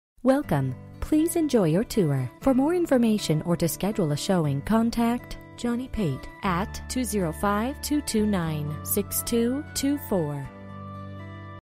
Homes for sale - 160 CREST DR, HUEYTOWN, AL 35023
GREAT CORNER LOT ON ONE OF HUEYTOWNS BUSIEST ROADS, ACROSS THE STREET FROM MICKEYS, NOT FR FROM RIVER SQUARE SHOPPING CENTER.  FRONTAGE ON ALLISON BONNETT DRIVE AND WEST CREST, PER TAX RECORDS 1.35 ACRES, GREAT LOCCATION FOR A FAST FOOD RESTURANT OR SMALL BUSINESS, HIGH TRAFFIC COUNT, SEWER IS AVAILABLE

Bedrooms: 0
Bathrooms: 0
Square Feet: 0
Price: $120,000